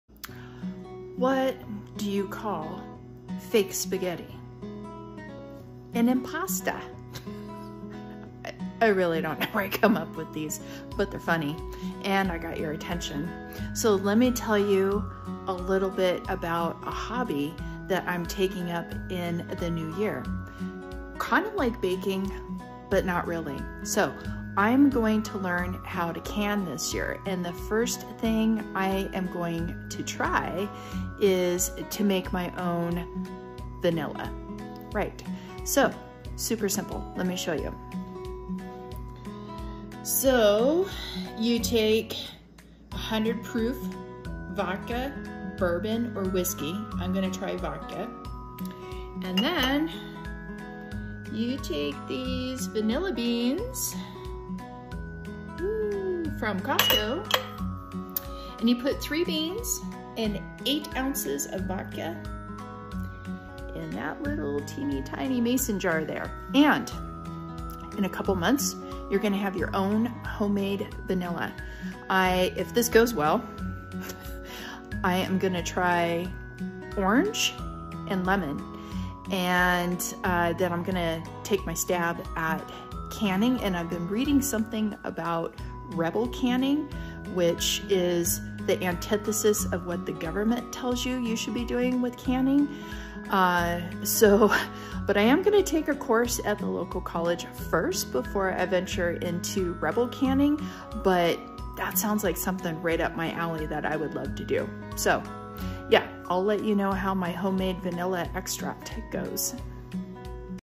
Think vanilla is plain? Wait until you hear about my daring culinary adventure.
What do you call fake spaghetti?

An  impasta!

I really don't know where I come up with these, but they're funny and I got your attention!

Let me tell you a little bit about a hobby that I'm taking up in the New Year.

Kinda like baking but not really.

I'm going to learn how to can this year.

The first thing I am going to try is to make my own vanilla extract.

So super simple!

You take 100 proof vodka, Bourbon or whiskey.

Then you take these vanilla beans from Costco.

You put three beans and 8 ounces of vodka in a Mason jar.

In a couple months you're gonna have your own homemade vanilla.

If this goes well, I am gonna try orange and lemon.

Then I'm gonna take my stab at canning.

I've been reading something about rebel canning, which is the antithesis of what the government tells
you you should be doing with canning.

I am gonna take a course at the local college first before I venture into Rebel Canning.

But that sounds like something right up my alley that I would love to do.

I'll let you know how my homemade vanilla extract goes.

→ If you like this content, follow me ⚓️Michelle Kimbro⚓️ (Efficiency Expert) I post daily at 7:00am MST.
→ Repost this post ♻️